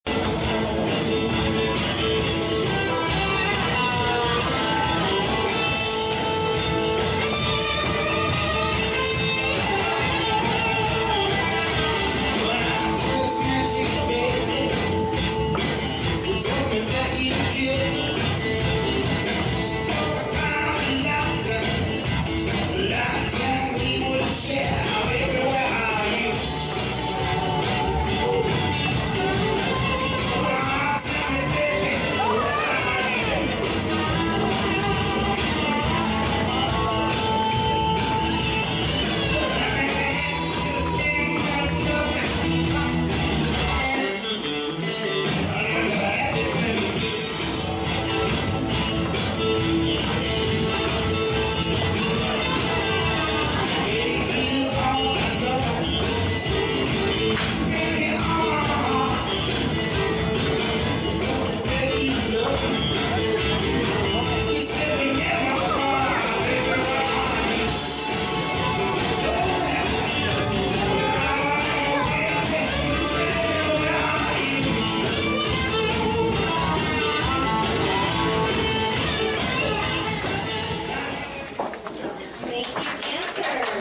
Holiday Mini Match 2019 - East Coast Swing 2
Style of dance: East Coast Swing
Type of dance: Heat
Level: Intermediate Silver
Dance Experience: 6 years

Instructor: Nora Jenkins
Event: 2019 Holiday Mini Match
Event Date: December 2019
Location: Bayou Dance Club, Pinellas Park, FL